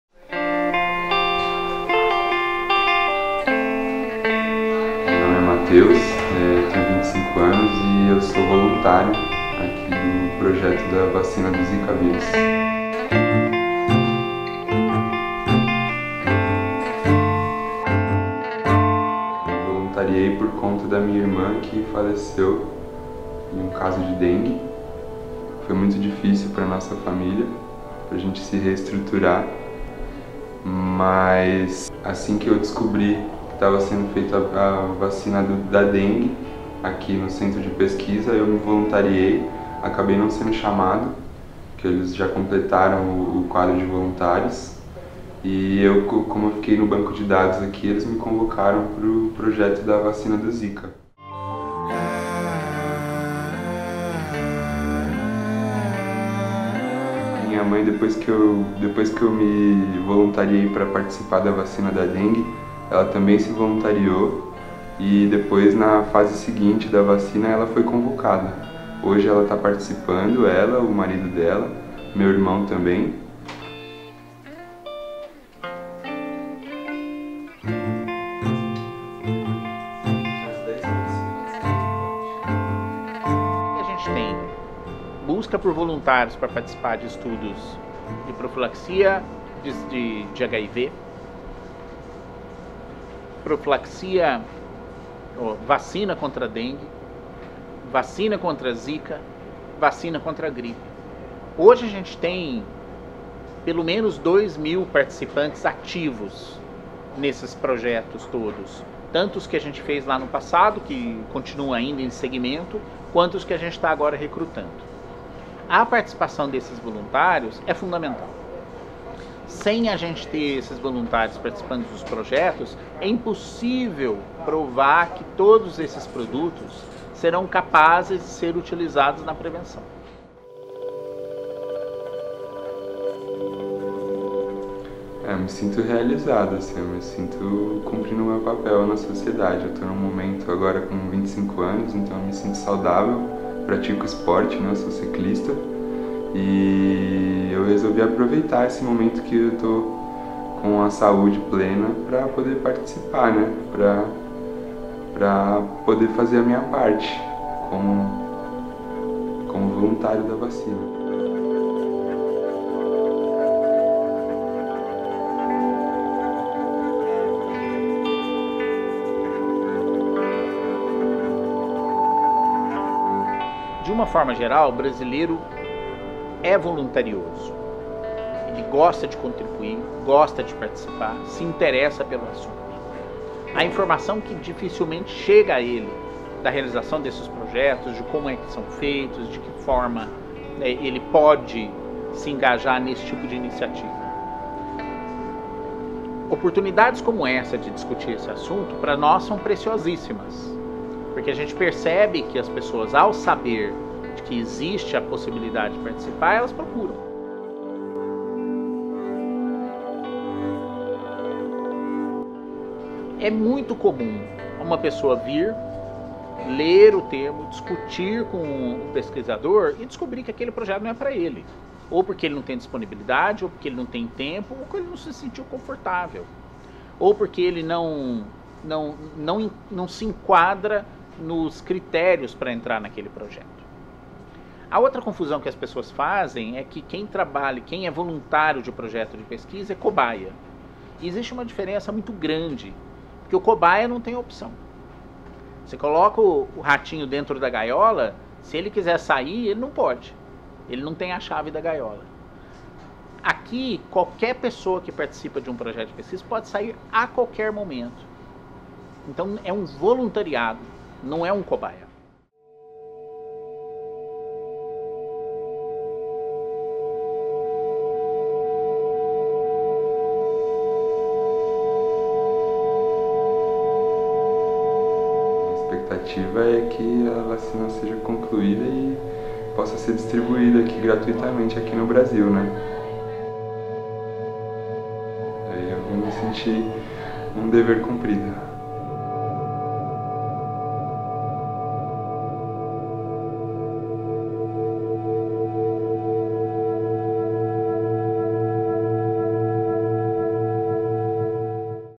Quem são os voluntários que testam vacinas no Brasil
Em 2017, ao menos 187 ensaios clínicos com medicamentos foram autorizados no país. E também duas vacinas, entre elas de Zika. Isso só foi possível porque voluntários aceitaram participar de testes com as vacinas -- a legislação brasileira não permite que essas pessoas recebam pela atividade. O que motiva alguém a se voluntariar de graça pela Ciência?